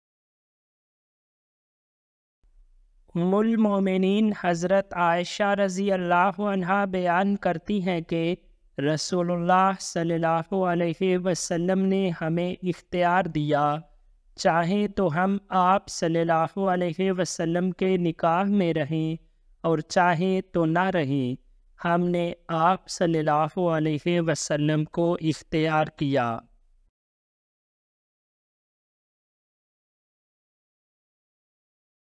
Tirmazi 1179 Hadith in Urdu رسول اللہ ﷺ نے اپنی ازواج کو کیا اختیار دیا تھا؟
ام المومنین حضرت عائشہ رضی اللہ عنہا بیان کرتی  ہیں کہ رسول اللہ ﷺ نے ہمیں اختیار دیا( چاہیں تو ہم آپ کے نکاح میں رہیں اور چاہیں تو نہ رہیں) ہم نے آپؐ کو اختیار کیا۔
حدیث جامع ترمذی الجامع الترمذي حديث